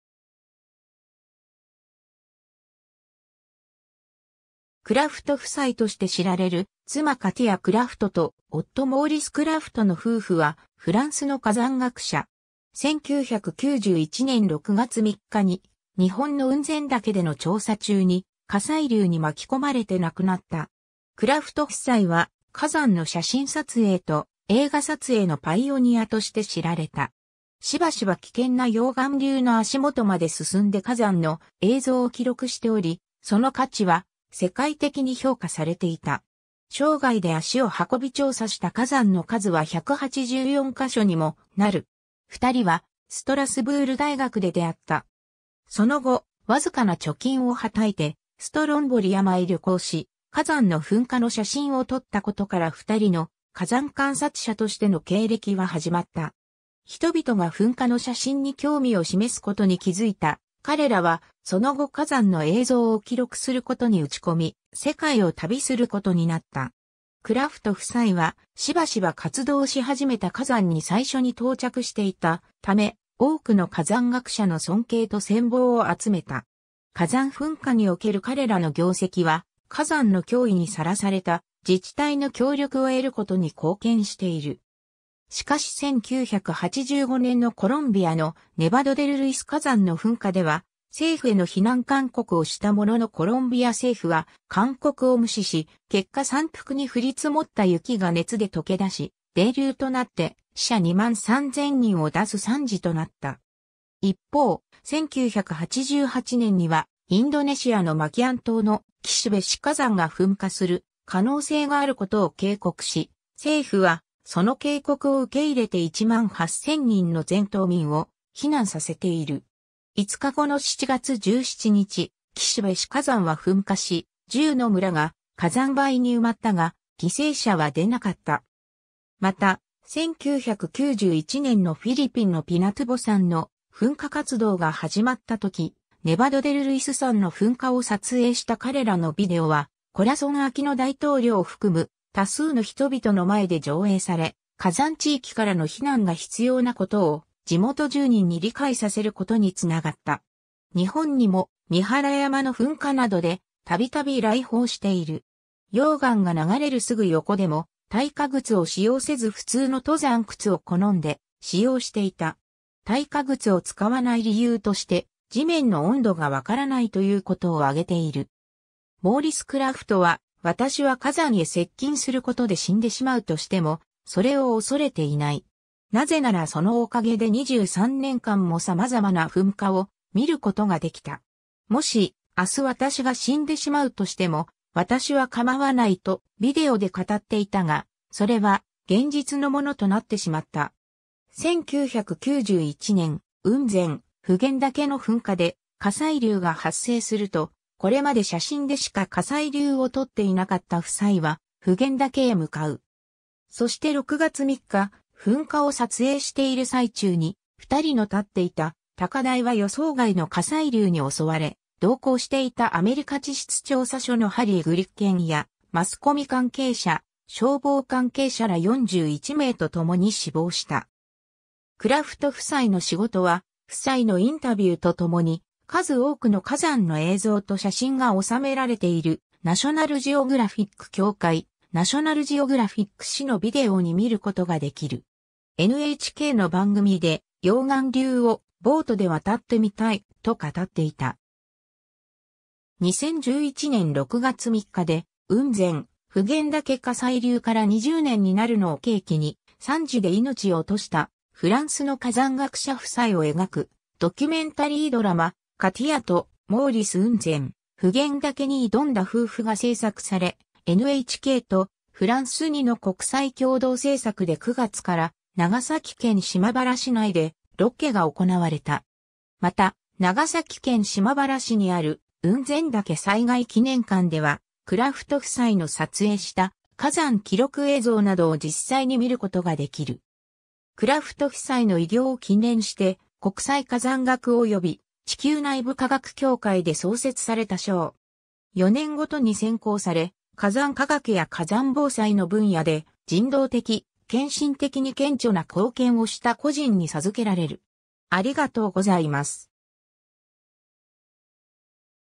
クラフト夫妻
オー＝ラン県出身の人物
クラフト夫妻（クラフトふさい）として知られる、妻カティア・クラフト（Katia Krafft、1942年4月17日 - 1991年6月3日）と夫モーリス・クラフト（Maurice Krafft、1946年3月25日 - 1991年6月3日）の夫婦は、フランスの火山学者。
1991年6月3日に、日本の雲仙岳での調査中に火砕流に巻き込まれて亡くなった。
クラフト夫妻は火山の写真撮影と映画撮影のパイオニアとして知られた。
しばしば危険な溶岩流の足もとまで進んで火山の映像を記録しており、その価値は世界的に評価されていた。
生涯で足を運び調査した火山の数は184か所にもなる。
 2人はストラスブール大学で出会った。
その後、わずかな貯金をはたいてストロンボリ山へ旅行し、火山の噴火の写真を撮ったことから2人の火山観察者としての経歴は始まった。
人々が噴火の写真に興味を示すことに気づいた彼らは、その後火山の映像を記録することに打ち込み、世界を旅することになった。
 クラフト夫妻は、しばしば活動し始めた火山に最初に到着していたため、多くの火山学者の尊敬と羨望を集めた。
火山噴火における彼らの業績は、火山の脅威にさらされた自治体の協力を得ることに貢献している。
しかし1985年のコロンビアのネバド・デル・ルイス火山の噴火では、政府への避難勧告をしたもののコロンビア政府は勧告を無視し、結果山腹に降り積もった雪が熱で溶けだし、泥流となって死者23000人を出す惨事となった。
一方、1988年にはインドネシアのマキアン島のキシ・ベシ火山が噴火する可能性があることを警告し、政府はその警告を受け入れて18000人の全島民を避難させている。
5日後の7月17日、キシ・ベシ火山は噴火し、10の村が火山灰に埋まったが犠牲者は出なかった。
また、1991年のフィリピンのピナトゥボ山の噴火活動が始まったとき、ネバド・デル・ルイス山の噴火を撮影した彼らのビデオは、コラソン・アキノ大統領を含む多数の人々の前で上映され、火山地域からの避難が必要なことを地元住人に理解させることに繋がった。
日本にも三原山の噴火などで、度々来訪している。
溶岩が流れるすぐ横でも、耐火靴を使用せず普通の登山靴を好んで使用していた。
耐火靴を使わない理由として、地面の温度が判らないということを挙げている。
 モーリス・クラフトは、「私は火山へ接近することで死んでしまうとしても、それを恐れていない。なぜならそのお陰で23年間も様々な噴火を見ることが出来た。もし明日私が死んでしまうとしても、私は構わない」とビデオで語っていたが、それは現実のものとなってしまった。
1991年、雲仙・普賢岳の噴火で火砕流が発生すると、これまで写真でしか火砕流を撮っていなかった夫妻は普賢岳へ向かう。
そして6月3日、噴火を撮影している最中に、2人の立っていた高台は予想外の火砕流に襲われ、同行していたアメリカ地質調査所のハリー・グリッケンやマスコミ関係者、消防関係者ら41名とともに死亡した。
 クラフト夫妻の仕事は、夫妻のインタビューとともに数多くの火山の映像と写真が収められているナショナルジオグラフィック協会『ナショナル・ジオグラフィック』誌のビデオに見ることができる。
NHKの番組で、溶岩流をボートで渡ってみたい、と語っていた。
2011年6月3日で雲仙・普賢岳火砕流から20年になるのを契機に、惨事で命を落としたフランスの火山学者夫妻を描くドキュメンタリードラマ「カティアとモーリス〜雲仙・普賢岳に挑んだ夫婦〜」が制作され、NHKとフランス2の国際共同制作で9月から長崎県島原市内でロケが行われた。
また、長崎県島原市にある雲仙岳災害記念館ではクラフト夫妻の撮影した火山記録映像などを実際に見ることができる。
 クラフト夫妻の偉業を記念して国際火山学及び地球内部化学協会で創設された賞。
4年ごとに選考され、火山科学や火山防災の分野で人道的・献身的に顕著な貢献をした個人に授けられる。